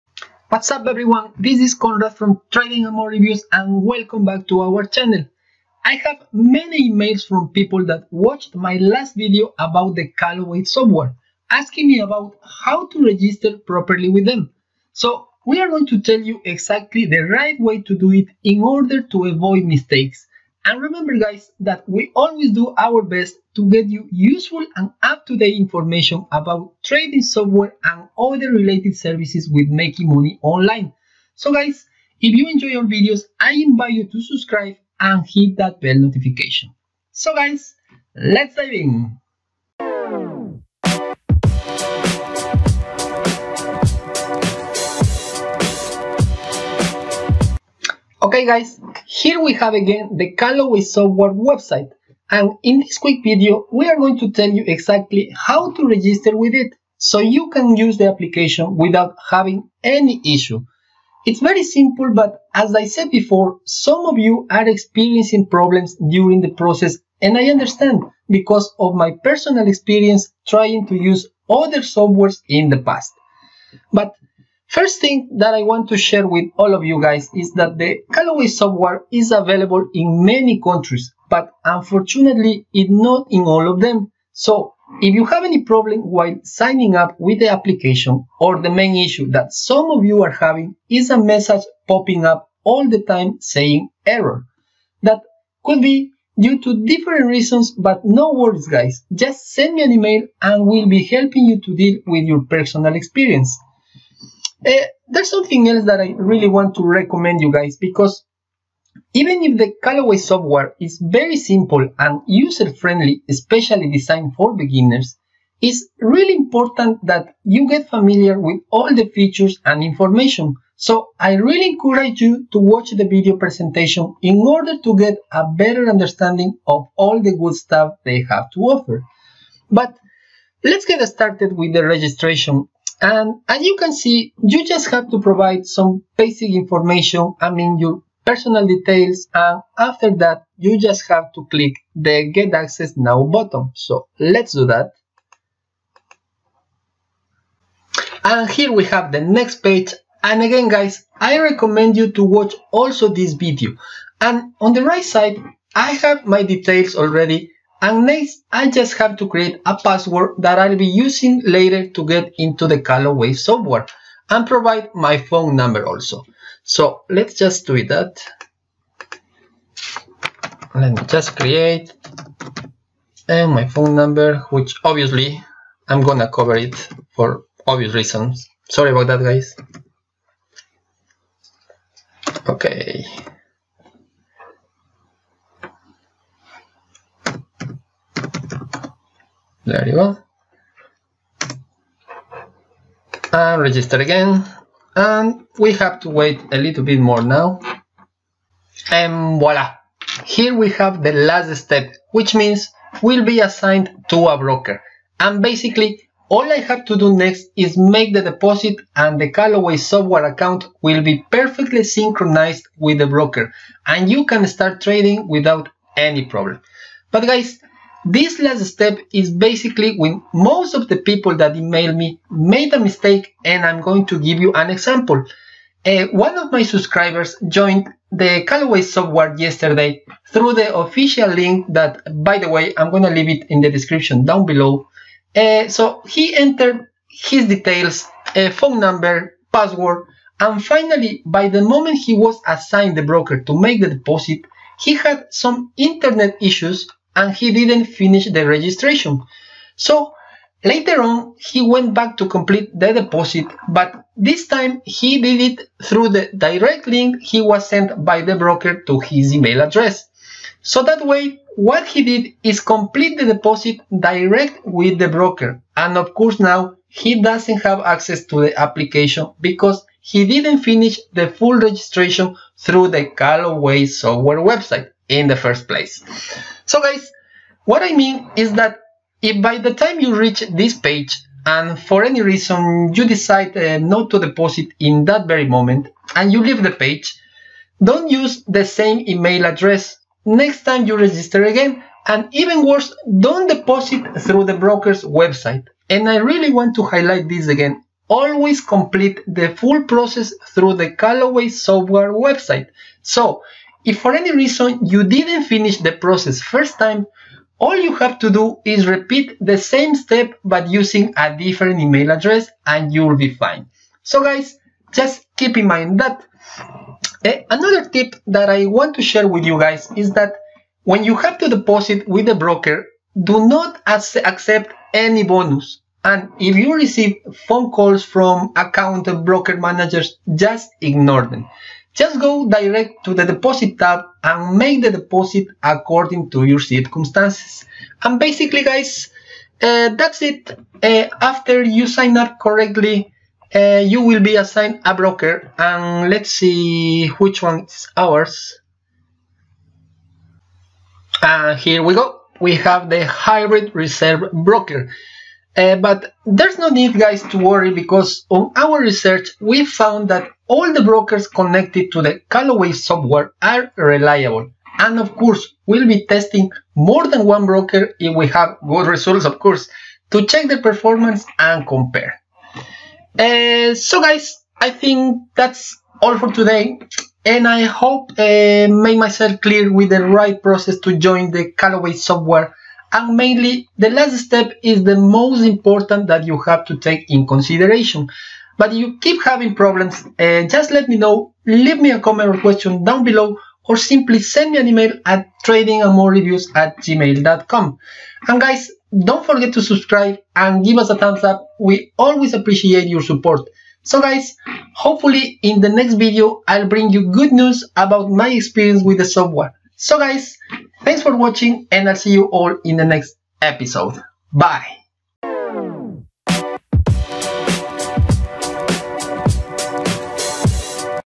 The Calloway Software-Sign Up Procedure-Common Mistakes
Today due to the many emails I had from people having problems with the registration to The Calloway Software, I'll explain in this video step by step the procedure in order for you to avoid mistakes and finish the sign up correctly.

I hope this video is helpful, if so give us a thumbs up and if you haven't subscribe yet, please go ahead and do it, any doubt please just leave me a comment or question down below.

Category
People & Blogs